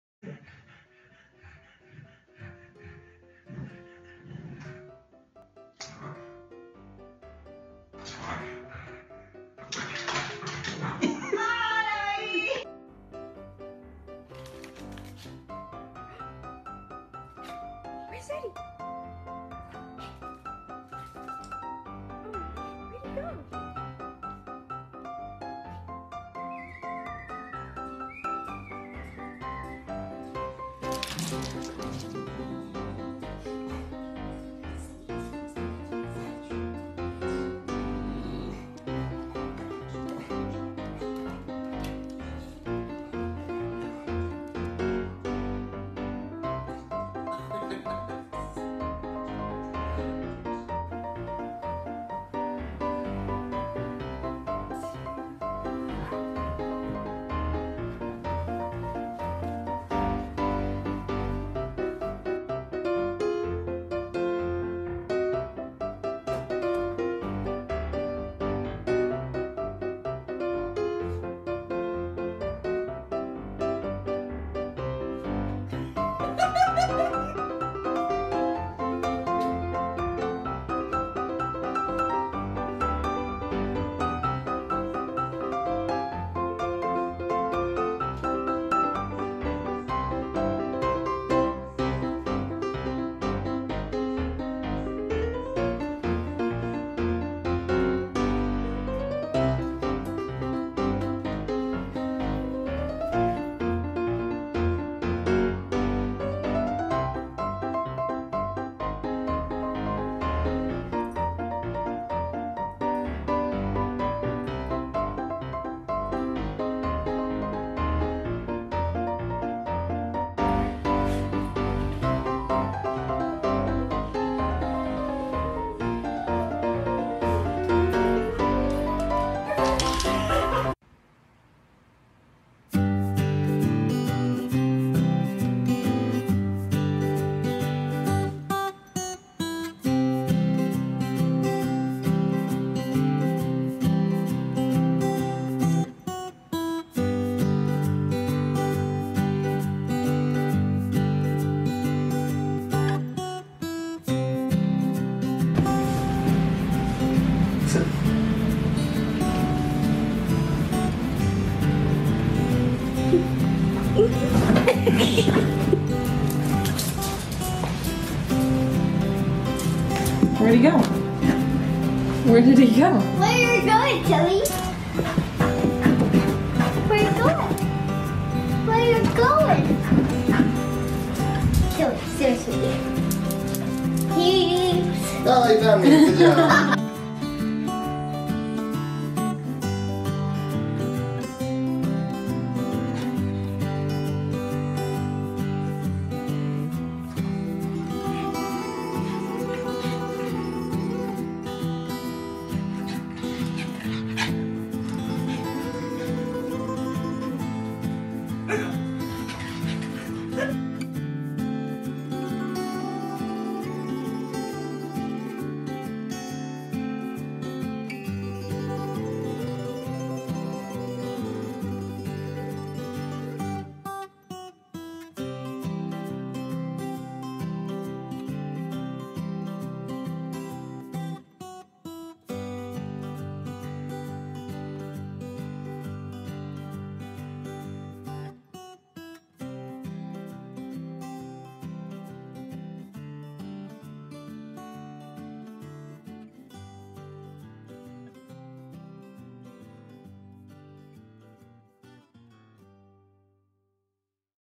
Fluff challenge with dogs - People tricking their dogs - Funny Dogs reaction
Fluff challenge with dogs.
Dogs funny reaction to disappearance of owners.